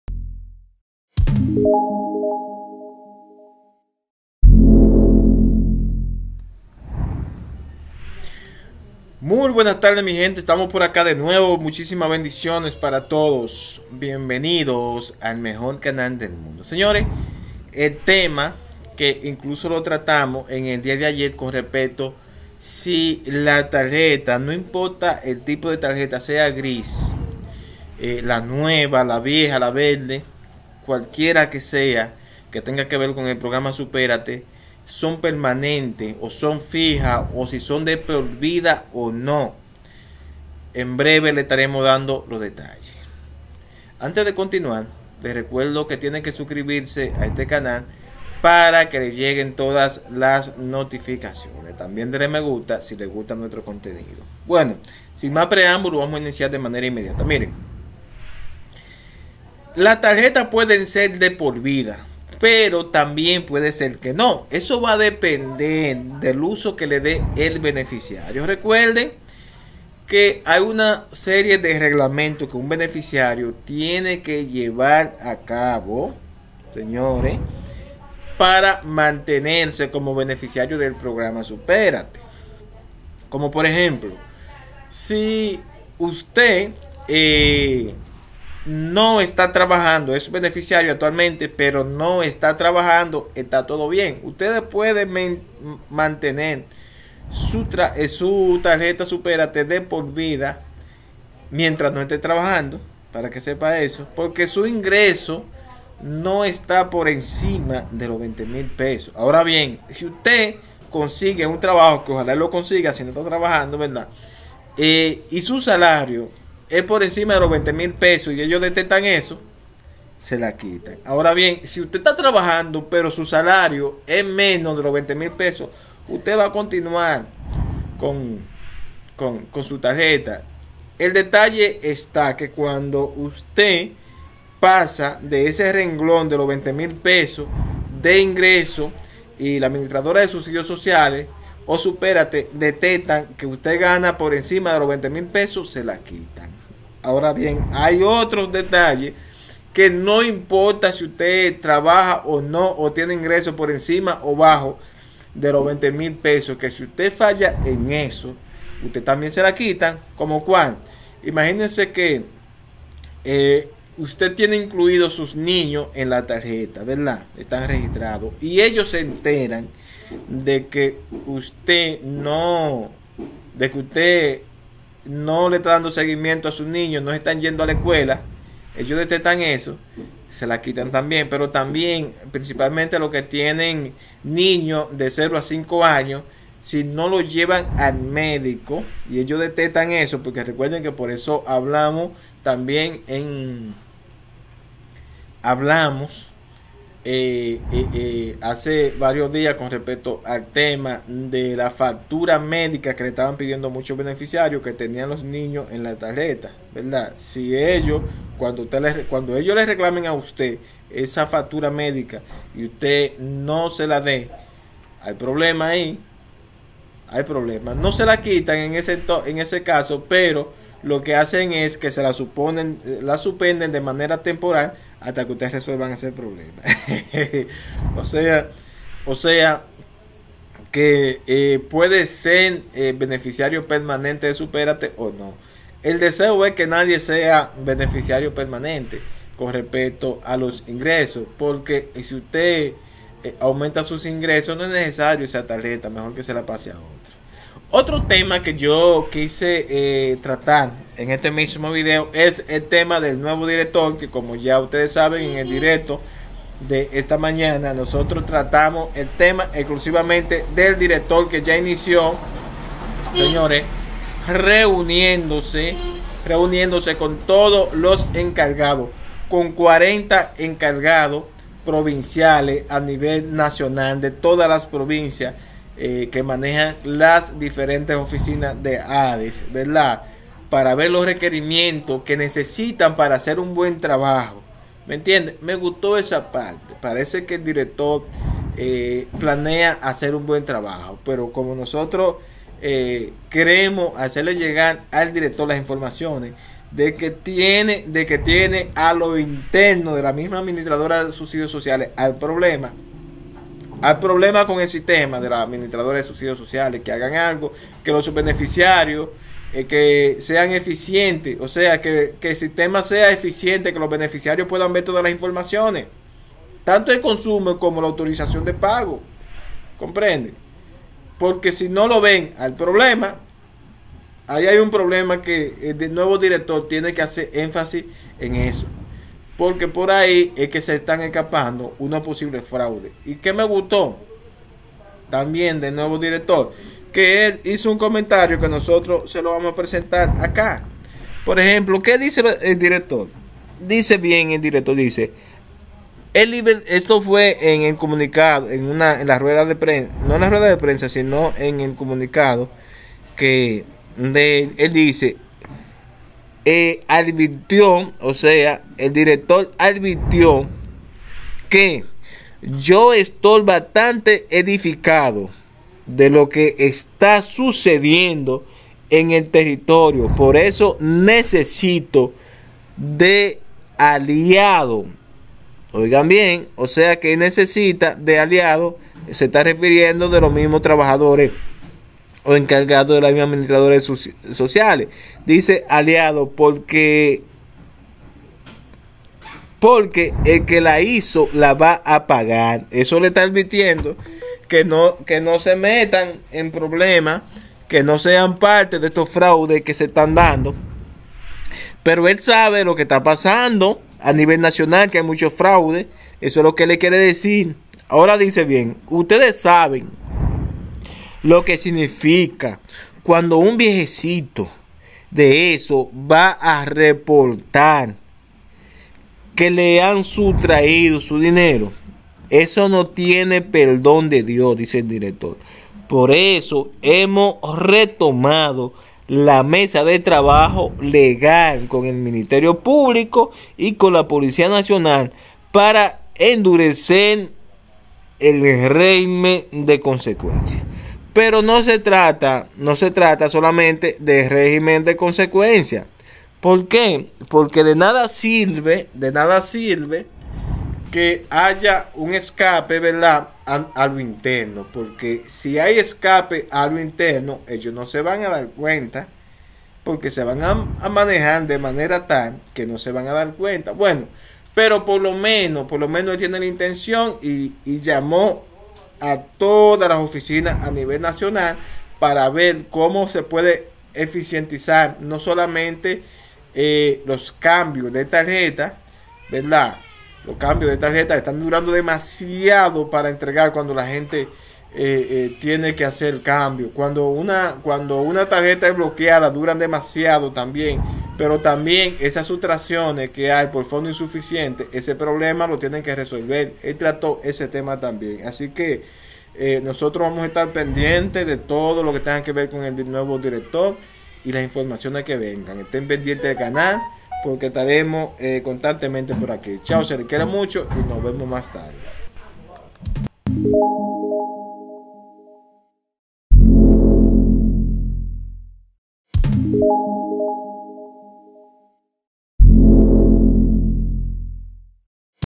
¿LA TARJETA ES DE POR VIDA O TEMPORAL?
Advirtió que: “Yo estoy bastante edificado de lo que está sucediendo en los territorios, por eso necesito de aliados, porque el que la hizo la va a pagar. ¿Ustedes saben lo que significa cuando un viejito de esos va a reportar que le han sustraído su dinero? Eso no tiene perdón de Dios, por eso hemos retomado la mesa de trabajo legal con el Ministerio Público y con la Policía Nacional para endurecer el régimen de consecuencias”.